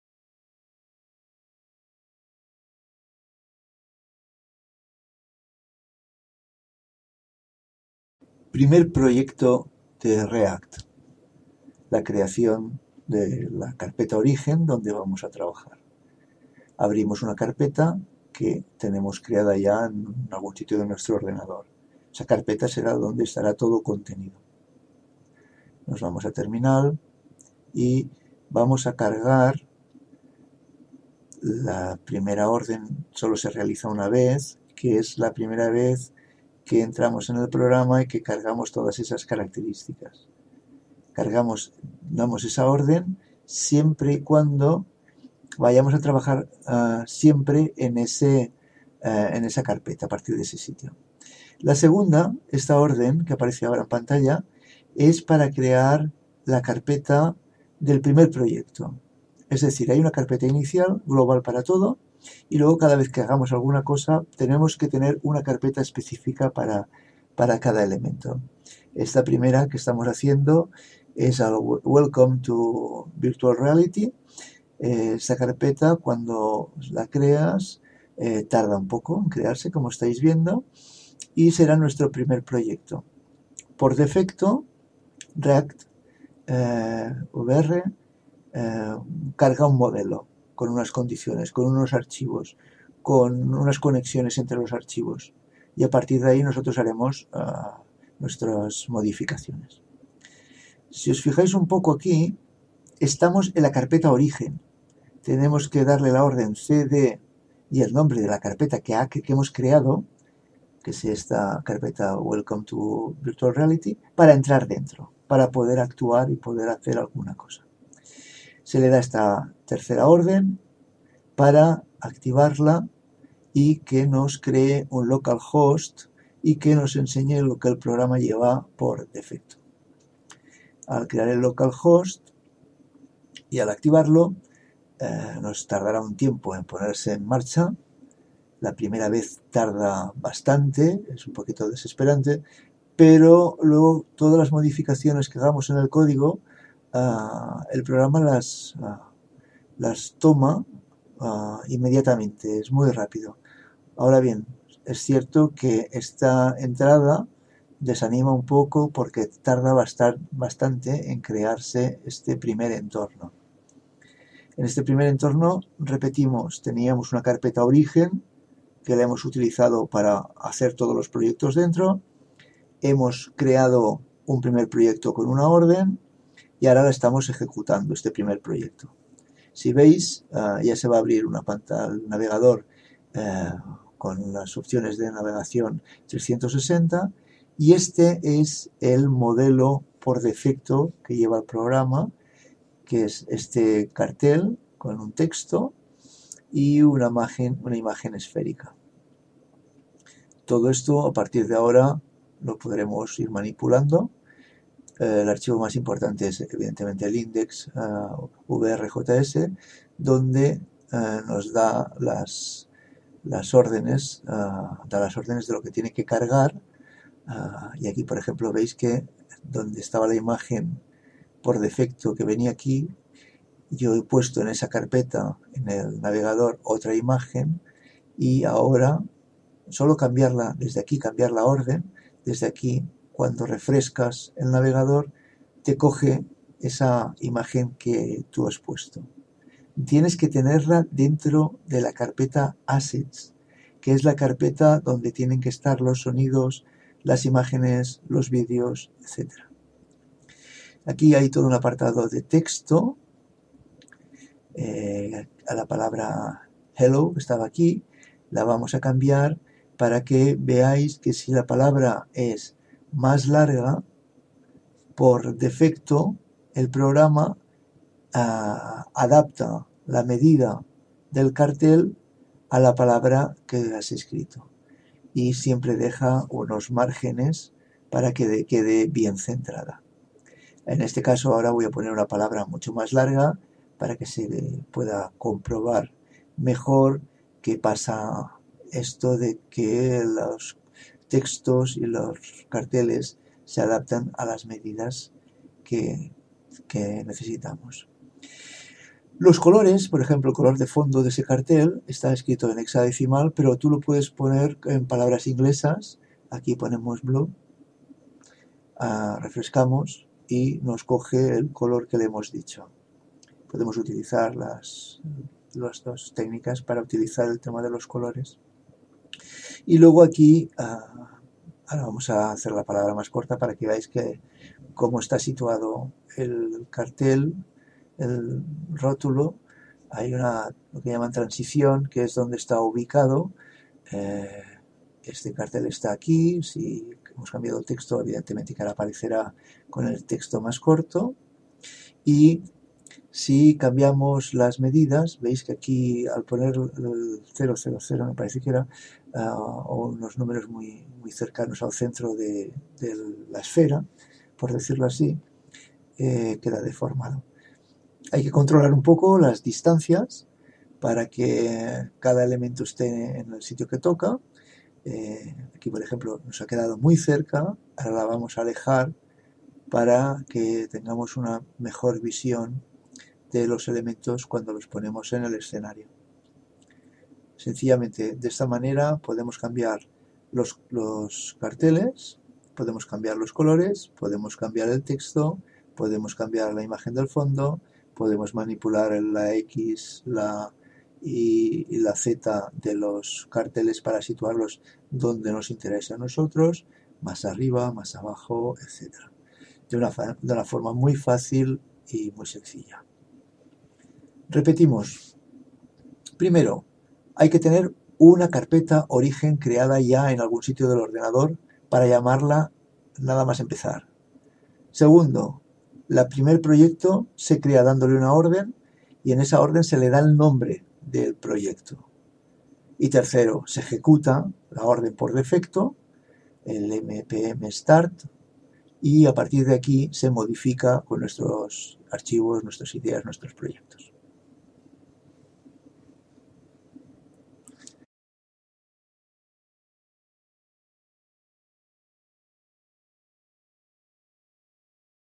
React 360 Hello Virtual Reality
React 360 Sound
Tutorials about ReactVR Spanish
Página con las informaciones necesarias para seguir este tutorial.
En algunos casos copiando las ordenes que se nos presenta.
Será conveniente tenerla abierta al principio para poder seguir mejor las indicaciones.